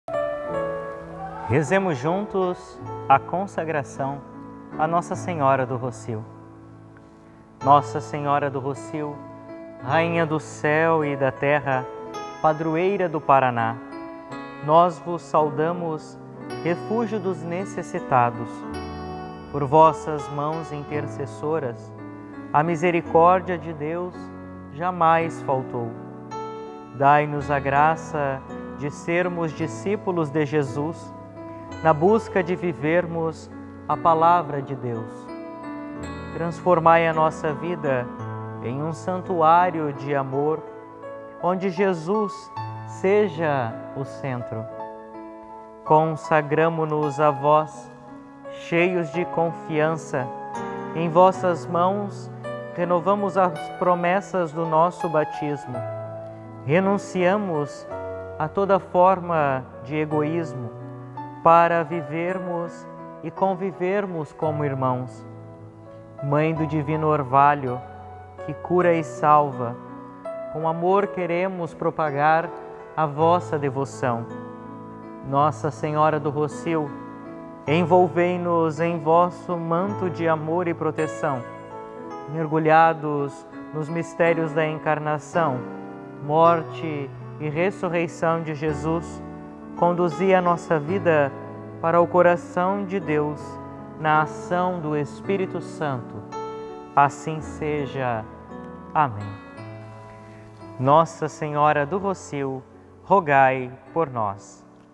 Consagração a Nossa Senhora do Rocio - Padroeira do Paraná.
CONSAGRAÇÃO A NOSSA SENHORA

Nossa Senhora do Rocio, Rainha do Céu e da Terra, padroeira do Paraná. Nós vos saudamos refúgio dos necessitados. Por vossas mãos intercessoras a misericórdia de Deus jamais faltou.
Dai-nos a graça de sermos discípulos de Jesus, na busca de vivermos a Palavra de Deus. Transformai a nossa vida em um santuário de amor, onde Jesus seja o centro.
Consagramo-nos a vós, cheios de confiança. Em vossas mãos renovamos as promessas do nosso Batismo; renunciamos a toda forma de egoísmo, para vivermos e convivermos como irmãos. Mãe do Divino Orvalho, que cura e salva, com amor queremos propagar a vossa devoção.
Nossa Senhora do Rocio, envolvei-nos em vosso manto de amor e proteção. Mergulhados nos mistérios da Encarnação, Morte e Ressurreição de Jesus, conduzi a nossa vida para o coração de Deus, na ação do Espírito Santo.
Assim seja, amém!